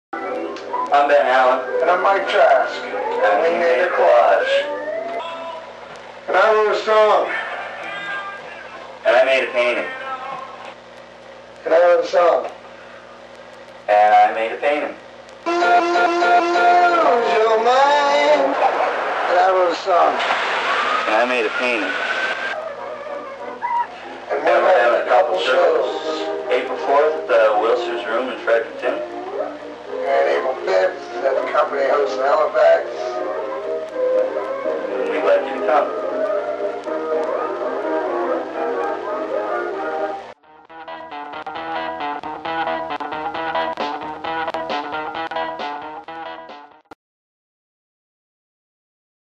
Mike Trask and Benjamin Allain Art Show
Mike Trask and Benjamin Allain began their collaborative art venture in December, 2012.

They first made a trip to Bargain Bob's second-hand junk shop, where they purchased a photo album belonging to a stranger. They used the photographs inside to make a collage.

Using the collage as inspiration, Mike wrote a tune...

Which Benjamin then listened to and created a painting...

This inspired another tune by Mike...

Which Ben then listened to and created another painting...

This cycle was completed three times.

Mike and Benjamin invite you to join them as they share their combined efforts with two shows. Mike will perform the tunes with a full band while Benjamin, propped up on a captain's chair, paints a fifth and final painting based on a new song that he will hear for the first time at the show. The other paintings will also be on display.

A project-package of sorts will be available for purchase at the shows, which will include a CD in a hand-made sleeve created from old photo album binders, and prints of all of the paintings.

Come and be a part of this unique artistic endeavour.